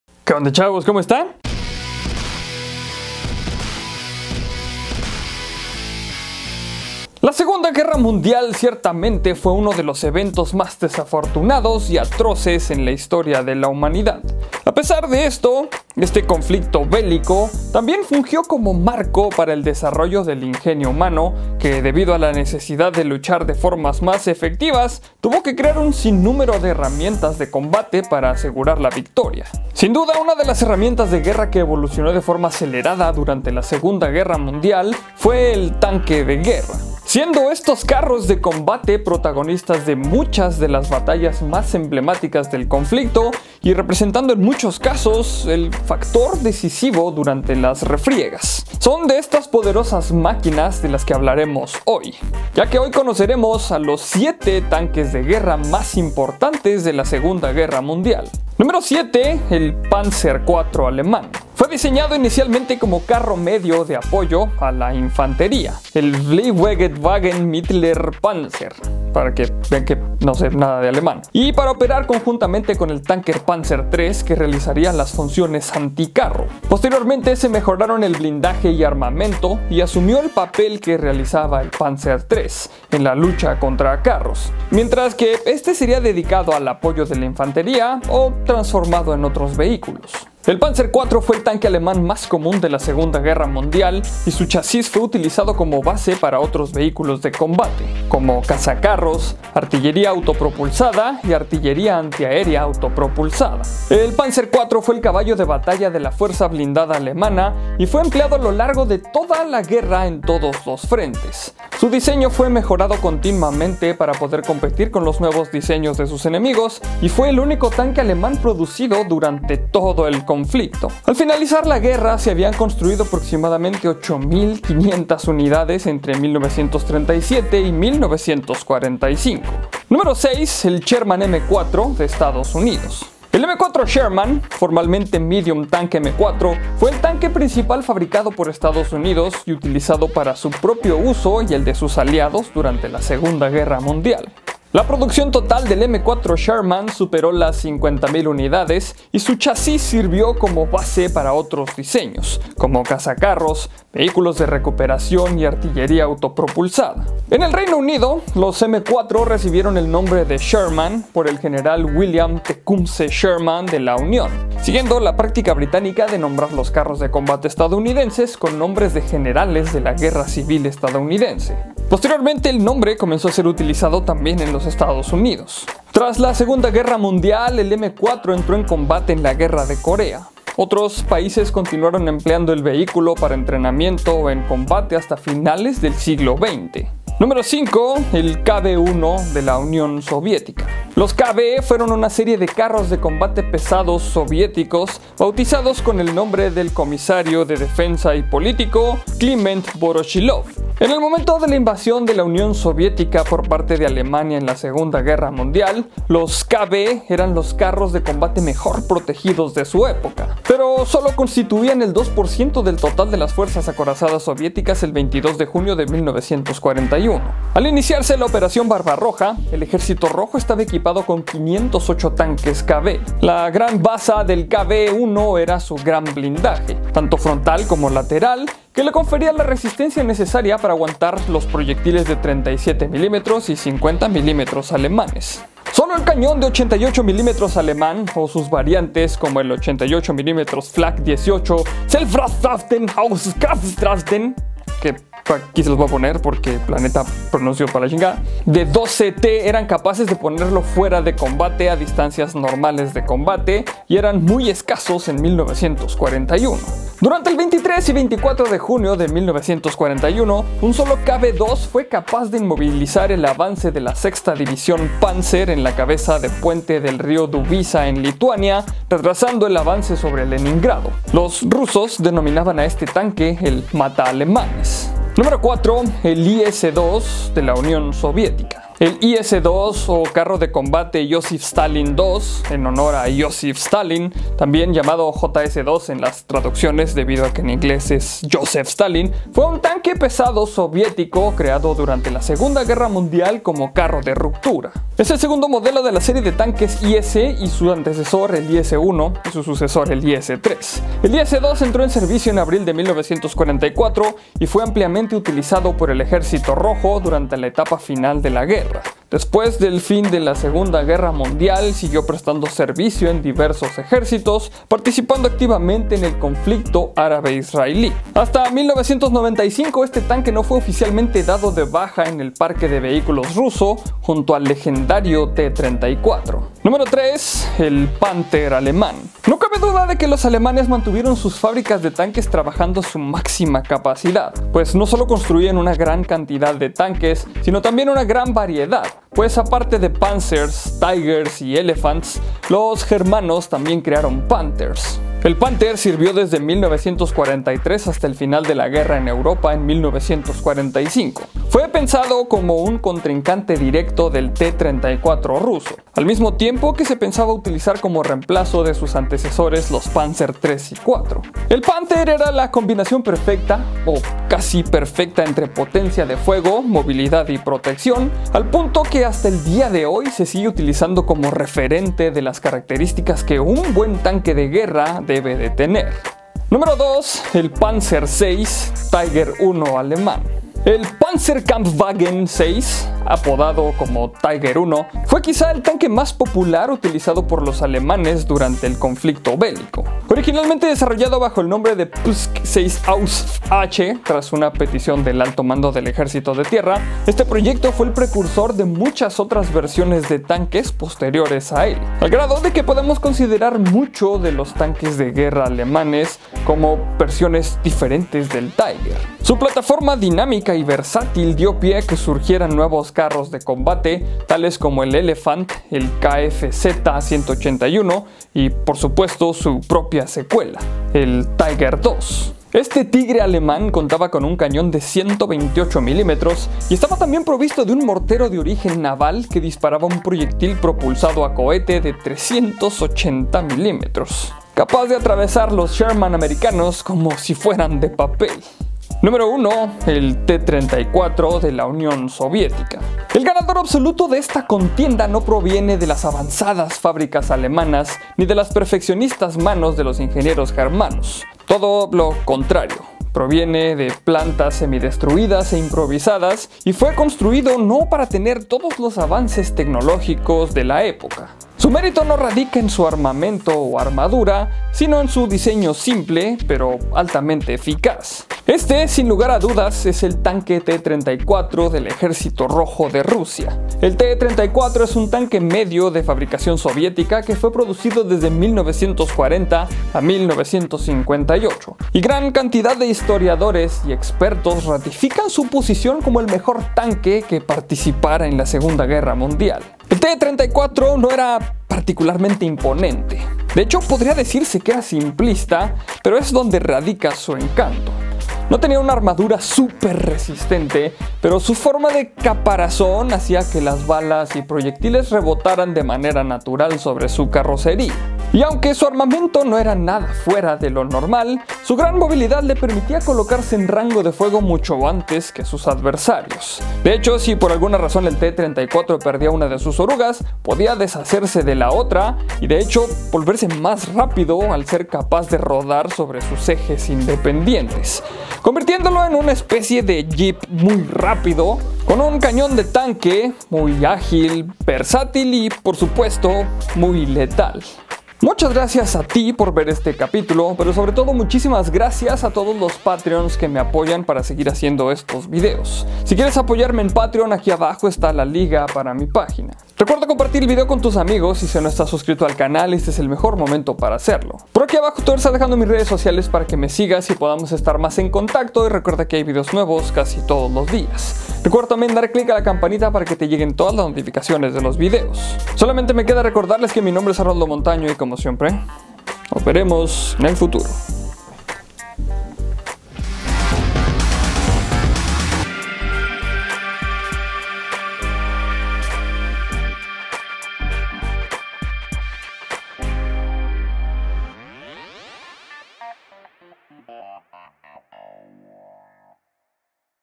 Los TANQUES más poderosos de segunda guerra mundial - Hey Arnoldo
Hoy daremos un paseo por la historia para revisar a los mejores tanques de guerra de la segunda guerra mundial, maquinas producto de la ingeniería que ganaron innumerables batallas en el frente.